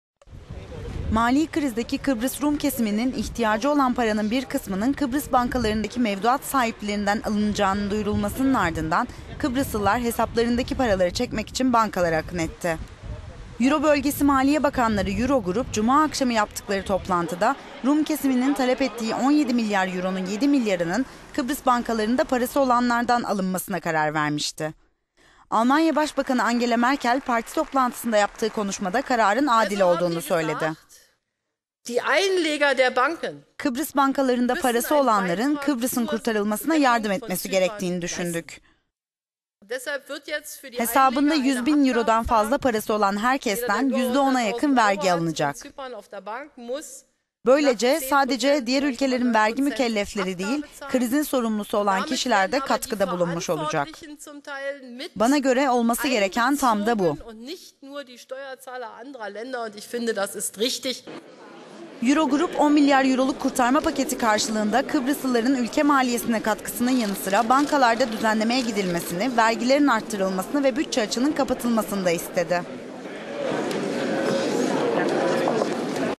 Kurtarma paketi şartlarını duyan Rumlar bankalara akın etti
Mali krizdeki Kıbrıs Rum Kesimi'ne verilecek kurtarma paketi karşılığında mevduat sahiplerinden yüzde 10 kesinti yapılacağını duyan Rumlar, hesaplarındaki paraları çekmek için bankalara akın etti.

Öte yandan, Kıbrıs Rum Kesimi Maliye Bakanı Michael Sarris, tek seferlik bir uygulama olacak kesintinin ülkeden sermaye kaçışına neden olmayacağını savundu.

Euro Bölgesi Maliye Bakanları (Eurogrup) Cuma akşamı yaptıkları toplantıda, Rum kesimin talep ettiği 17 milyar Euro'nun 7 milyarının Kıbrıs bankalarında parası olanlardan alınmasına karar vermişti.

Almanya Başbakanı Angela Merkel, parti toplantısında yaptığı konuşmada, kararın adil olduğunu söyledi: "Kıbrıs bankalarında parası olanların ülkenin kurtarılmasına yardım etmesi gerektiğini düşündük. Hesabında 100 bin Euro'dan fazla parası olan herkesten yüzde 10'a yakın vergi alınacak. Böylece, sadece diğer ülkelerin vergi mükellefleri değil, krizin sorumlusu olan kişiler de katkıda bulunmuş olacak. Bence, olması gereken tam da bu."

Eurogrup, kurtarma paketi karşılığında, Kıbrıslıların ülke maliyesine katkısının yanı sıra bankalarda düzenlemeye gidilmesini, vergilerin arttırılmasını ve bütçe açığının kapatılmasını da istedi.

Avrupa Birliği'nden 10 milyar Euro'luk kredi alacak Kıbrıs, bir yandan da 2011 yılında Rusya'dan aldığı 2.5 milyar Euro kredinin geri ödemesinin 5 yıl daha uzatılması ve faiz oranının düşürülmesi için Moskova'yla pazarlık yapıyor.